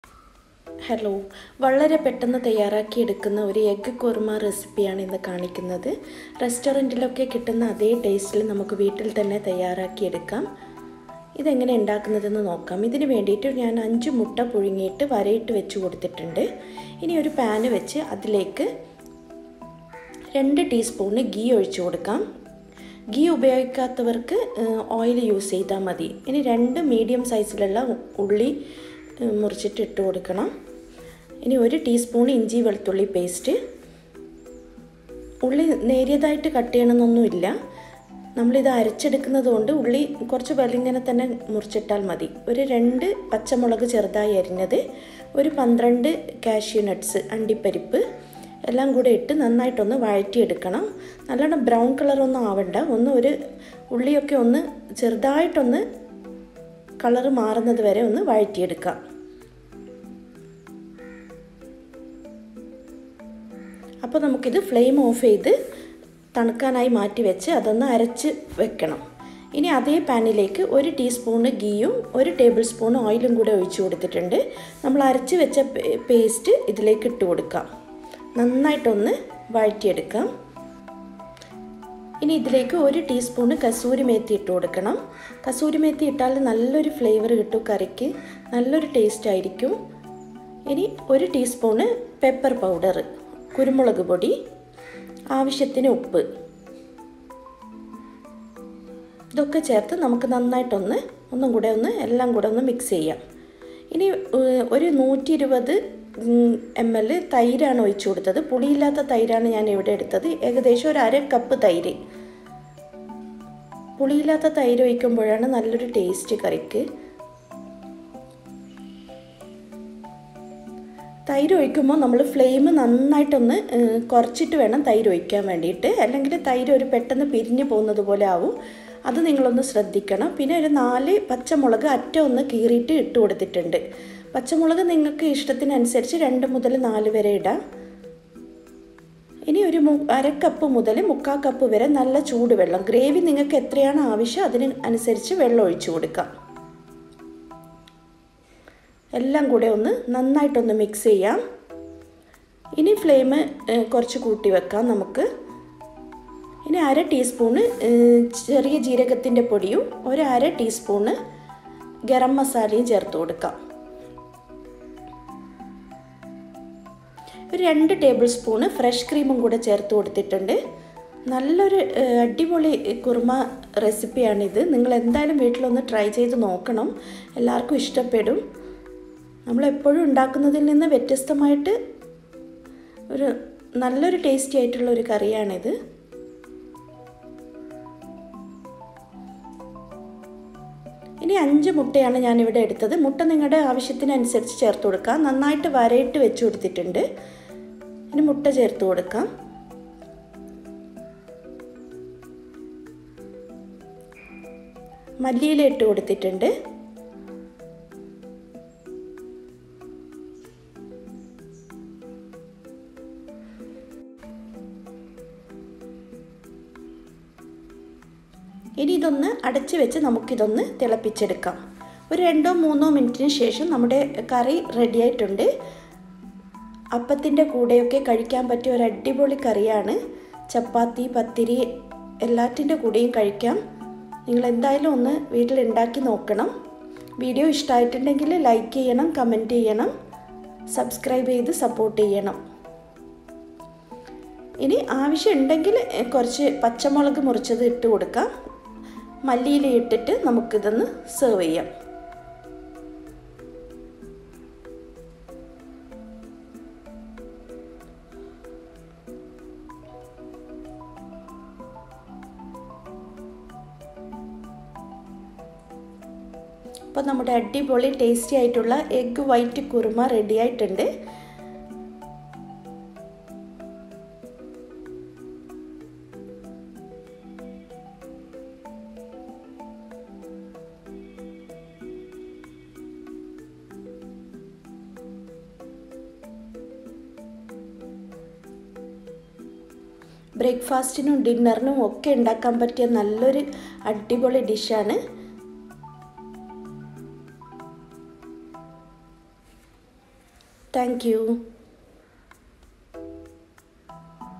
ഒരു വെറൈറ്റി മുട്ട- വൈറ്റ് കുറുമ
eggboiled -5
ghee -2tsp
onion-2 (medium)
green chillies-2chopped+4whole chillies
cashewnuts -12
gingergarlicpaste-1tsp
kasurimethi(dried fenugreek leaves)-1tsp
pepperpowder-1tsp
yogurt-120 ml(1/2 cup)
hotwater _1/2 to 3/4 cup
freshcream-2tbsp
cuminpowder-1/2tsp
garammasala -1/2tsp